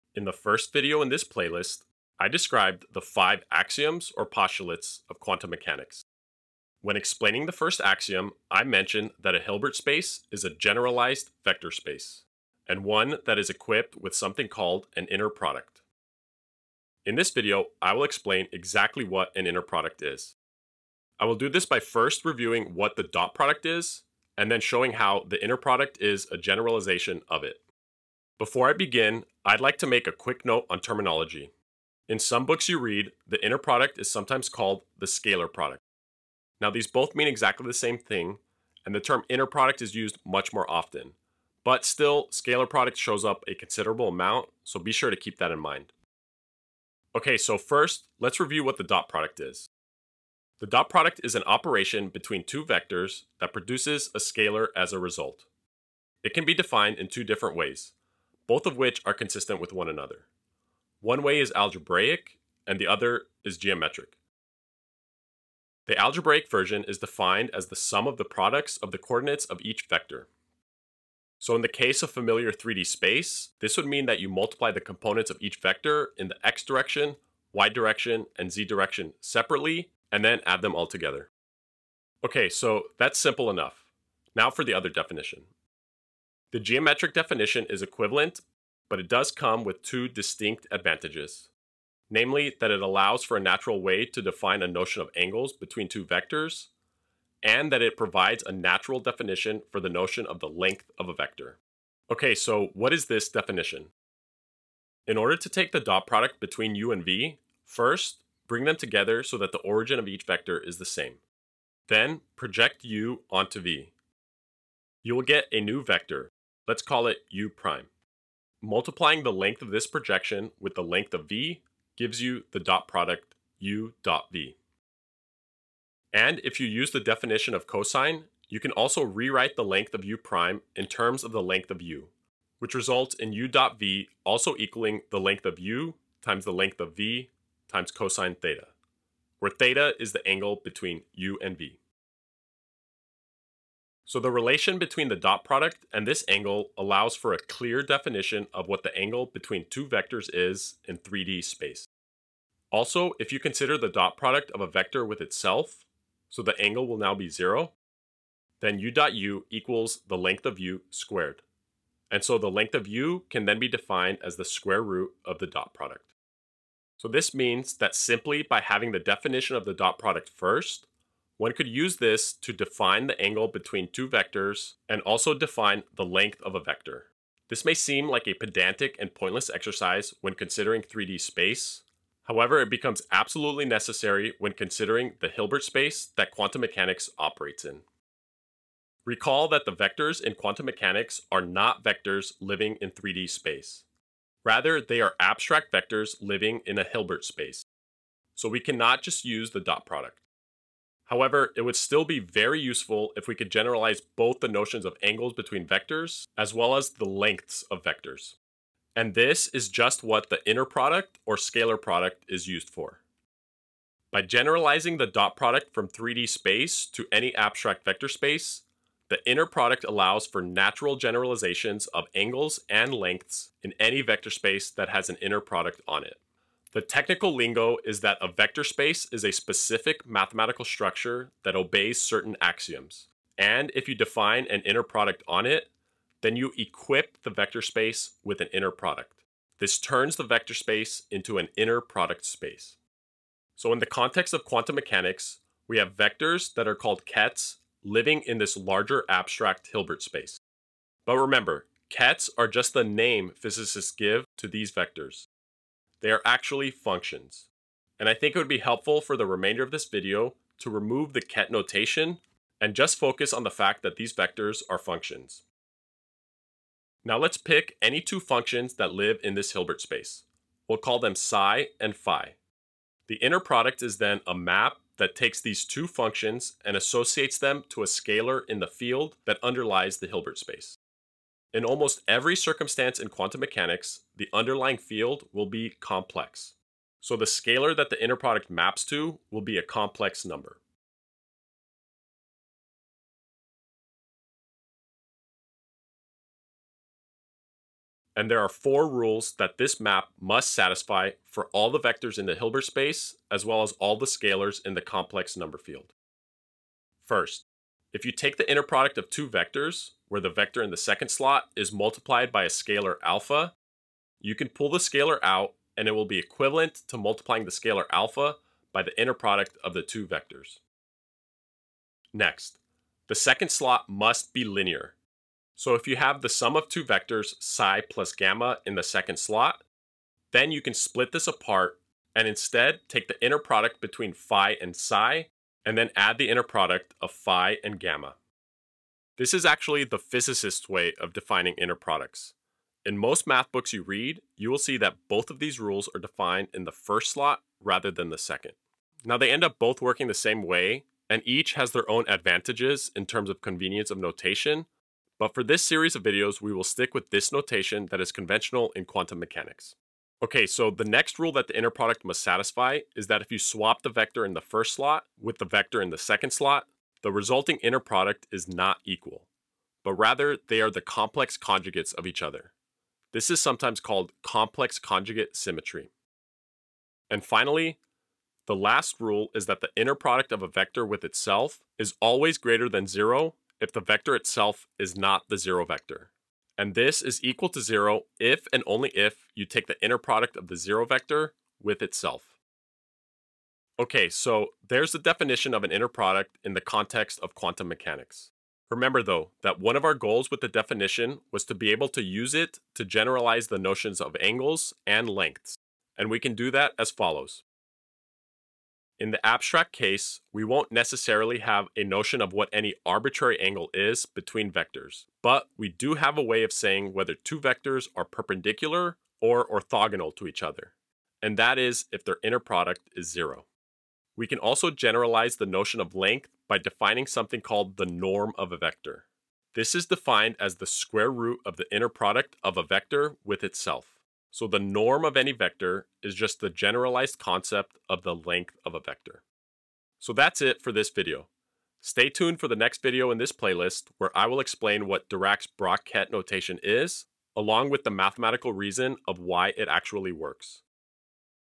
Inner Products | Pivotal Math of Quantum Mechanics | Quantum Theory
This is the third video in my Quantum Theory playlist. I explain what the inner product is in the context of Quantum Mechanics. I do this by reviewing what the dot product is and showing how the inner product is a generalization of it.

0:00 Introduction
0:49 Review of the Dot Product
1:05 Algebraic Definition of the Dot Product
1:24 Geometric Definition of the Dot Product
2:24 Relation between Dot Product and Angles/Lengths of Vectors
3:09 Motivation of Inner Product in Quantum Mechanics
4:57 The 4 Rules an Inner Product Must Satisfy (Definition)
6:33 Conclusion

If you have any questions or suggestions for future videos, please leave a comment here or shoot me an email at .